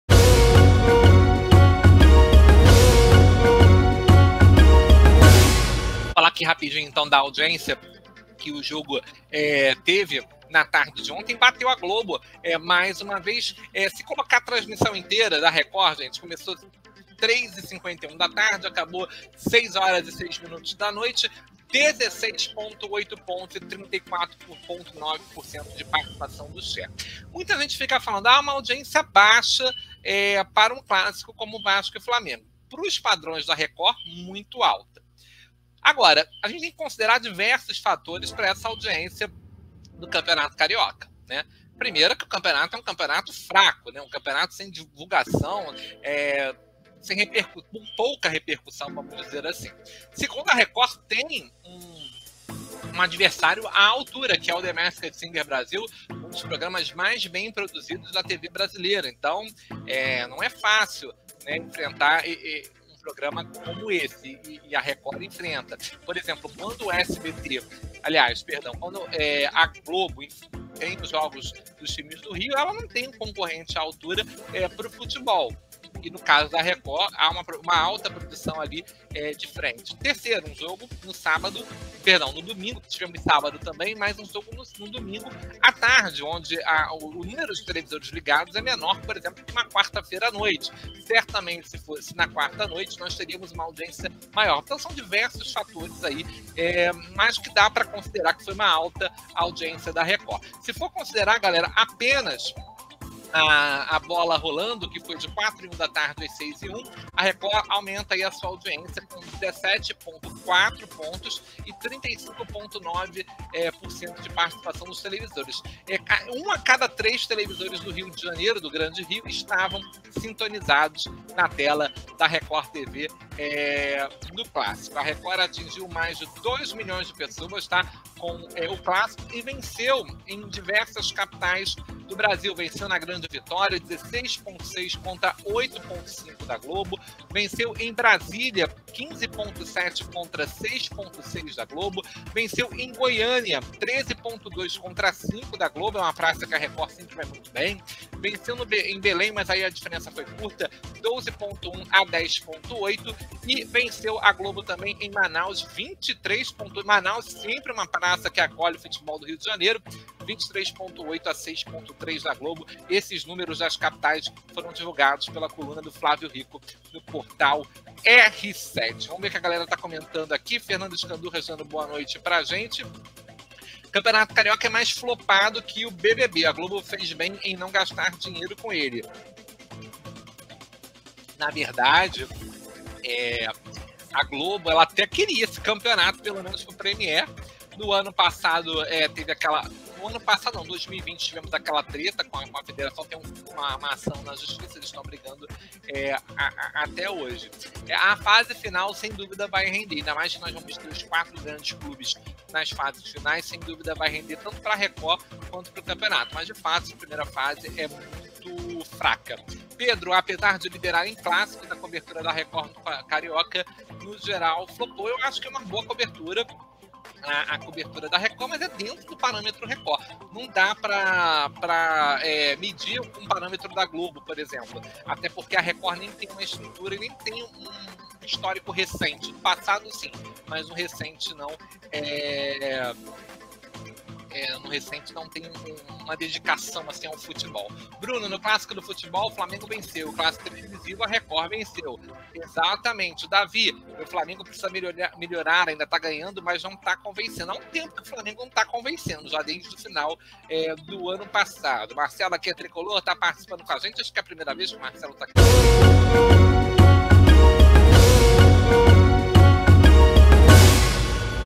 AUDIÊNCIA: RECORD VENCE A GLOBO E CHEGA À LIDERANÇA COM FLAMENGO X VASCO
A transmissão Flamengo x Vasco, pelo Campeonato Carioca, garantiu a liderança para a Record TV neste domingo, 6 de março.

Matérias relacionadas:
Record lidera com Flamengo x Vasco pelo Carioca. Confira os números de audiência